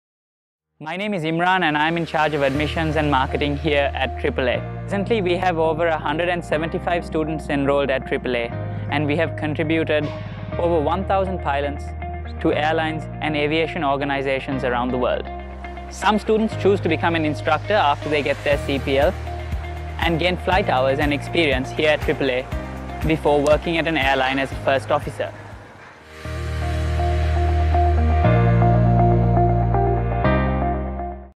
Life at Asian Academy of Aeronautics - Maldives
Founded in 1991, AAA has steadily grown to become the largest flight training academy in South Asia. Through efficient, highly focused training towards commercial pilot certification, AAA prepares men and women for professional pilot careers with airlines, corporate flight departments, air charter companies and flight schools.